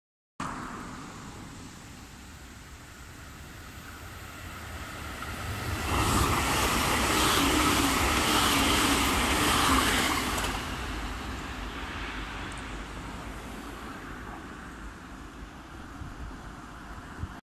130kmオーバー？特急ソニック超高速通過
窓見ればわかりますが
130kmオーバーしてるのでは、